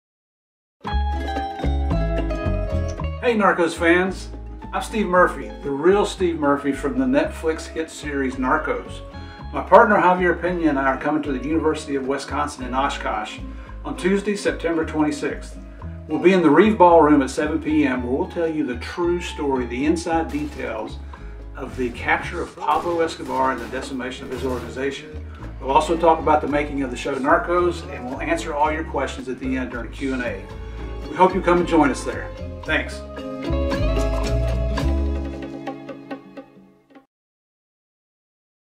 CHASING PABLO - WORLD TOUR: University of Wisconsin, Oshkosh
CHASING PABLO: WORLD TOUR
The True Story of Pablo Escobar

The University of Wisconsin, Oshkosh
Reeve Union Ballroom
September 26, 2017 at 7pm
You may have seen TV shows and documentaries, or read books about the world’s wealthiest, and most violent drug trafficker, but now you have the opportunity to hear the real story of what happened in the investigation of Pablo Escobar and the Medellin Cartel. DEA special agents Javier Peña and Steve Murphy were assigned as the lead investigators targeting Escobar and his organization.

These true American heroes provide a lesson in history as they discuss their efforts bringing down the world’s first narco-terrorist, the challenges they faced in oftentimes hostile and life-threatening environments, and the innovative strategies they employed to successfully end the reign of terror of the world’s most wanted criminal.

Their presentation is followed by a Q&A opportunity during which Javier and Steve will entertain questions related to their investigation, the making of Narcos and more.